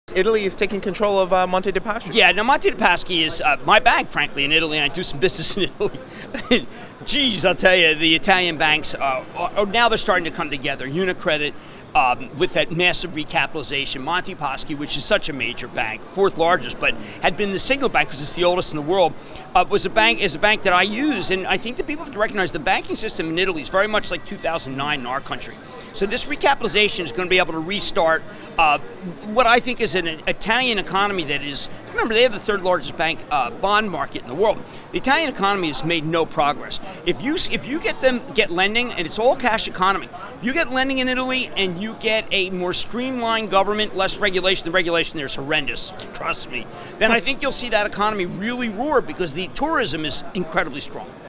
Jim Cramer: Italy's Banks Are Like Ours Were Back in 2009
Amid Italy's takeover of Monte dei Paschi di Siena BMDPY , TheStreet's Jim Cramer said the banks in Italy are similar to U.S. banks back in 2009.

Cramer said improved lending and less regulation in Italy will boost its economy.